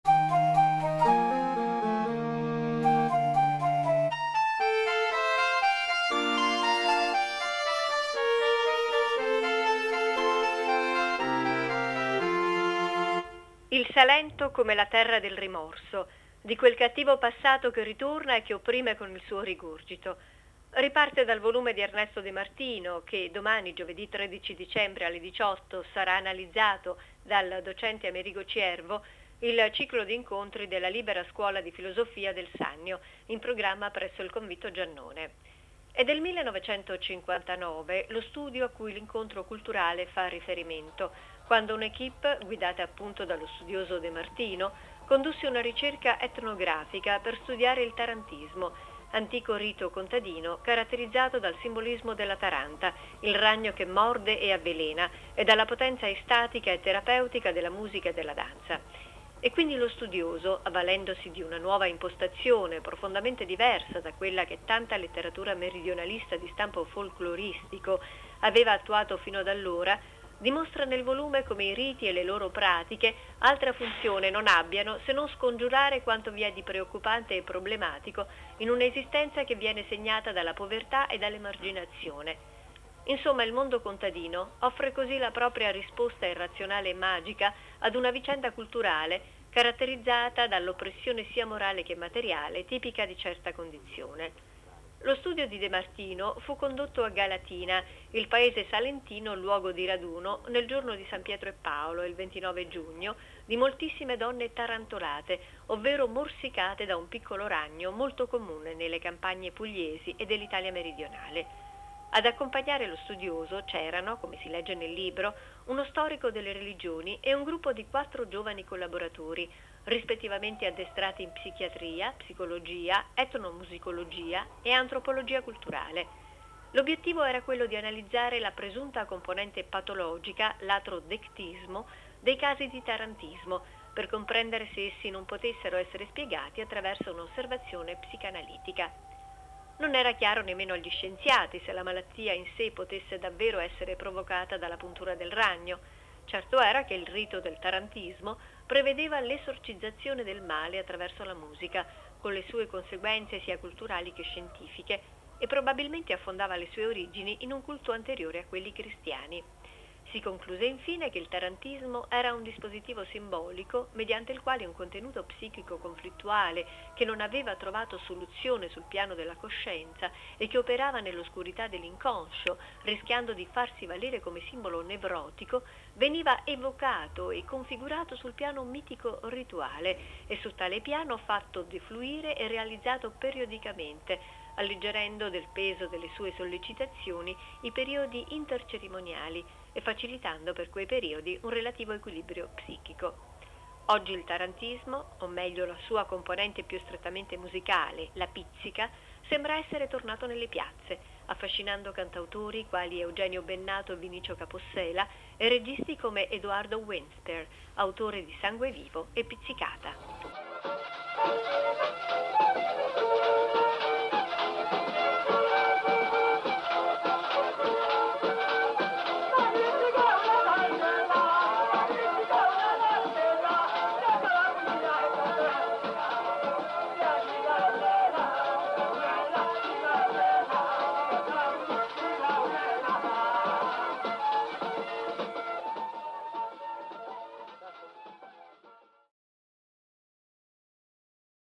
ELLE TV - Palcheventi: Il "tarantismo" ne "La terra del rimorso" di Ernesto De Martino
Palcheventi: rubrica culturale curata da Maria Ricca.
Domani, 13 dicembre, nell'ambito del ciclo di incontri della Libera Scuola di Filosofia al Convitto: il "tarantismo" ne "La terra del rimorso" di Ernesto De Martino: ne parla Amerigo Ciervo.
Il Salento come "La terra del rimorso", di quel "cattivo passato che ritorna e che opprime con il suo rigurgito". Riparte dal volume di Ernesto De Martino, che, giovedì 13 dicembre, alle 18, sarà analizzato dal docente  Amerigo Ciervo, il ciclo di incontri della Libera Scuola di Filosofia del Sannio, in programma presso il Convitto Giannone.
E' del 1959 lo studio a cui l'incontro culturale fa riferimento, quando un'équipe, guidata appunto dallo studioso  De Martino, condusse una ricerca etnografica per studiare il "tarantismo", antico rito contadino caratterizzato dal simbolismo della taranta - il ragno che morde e avvelena - e dalla potenza estatica e terapeutica della musica e della danza.
E quindi lo studioso, avvalendosi di una nuova impostazione, profondamente diversa da quella che tanta letteratura meridionalista di stampo folcloristico aveva attuato fino ad allora, dimostra nel volume come i riti e le loro pratiche altra funzione non abbiano se non scongiurare quanto vi è di preoccupante e problematico in un'esistenza che viene segnata dalla povertà e dall'emarginazione. Insomma il mondo contadino offre così la propria risposta irrazionale e magica ad una vicenda culturale carattterizzata dall'oppressione sia morale che materiale, tipica di certa condizione.
Lo studio di De Martino fu condotto a  Galatina, il paese salentino luogo di raduno, nel giorno di San Pietro e Paolo, il 29 giugno, di moltissime donne "tarantolate", ovvero morsicate da un piccolo ragno molto comune nelle campagne pugliesi e dell'Italia Meridionale. Ad accompagnare lo studioso c'erano, come si legge nel libro, " uno storico delle religioni e un gruppo di quattro giovani collaboratori, rispettivamente addestrati in psichiatria, psicologia, etnomusicologia e antropologia culturale". L'obiettivo era quello di analizzare la presunta componente patologica (latrodectismo) dei casi di tarantismo, per comprendere se essi non potessero essere spiegati attraverso una osservazione psicanalitica.
Non era chiaro, nemmeno agli scienziati, se la malattia in sé potesse davvero essere provocata dalla  puntura del ragno. Certo era che il rito del "tarantismo" prevedeva  l'esorcizzazione del male attraverso la musica, con le sue conseguenze sia culturali che scientifiche e probabilmente affondava le sue origini in un culto anteriore a quelli cristiani. Si concluse, infine, che il tarantismo era "un dispositivo simbolico mediante il quale un contenuto psichico conflittuale che non aveva trovato soluzione sul piano della cosicenza, e che operava nell'oscurità dell'inconscio, rischiando di farsi valere come simbolo nevrotico, veniva evocato e configurato sul piano mitico-rituale, e su tale piano fatto defluire e e realizzato periodicamente, alleggerendo del peso delle sue sollecitazioni i periodi intercerimoniali e facilitando, per quei periodi, un relativo equilibrio psichico."
Oggi il tarantismo o, meglio, la sua componente più strettamente musicale, la pizzica, sembra essere tornato nelle piazze, affascinando cantautori quali Eugenio Bennato e Vinicio Capossela e registi come  Edoardo Winspeare, autore di  "Sangue vivo" e "Pizzicata".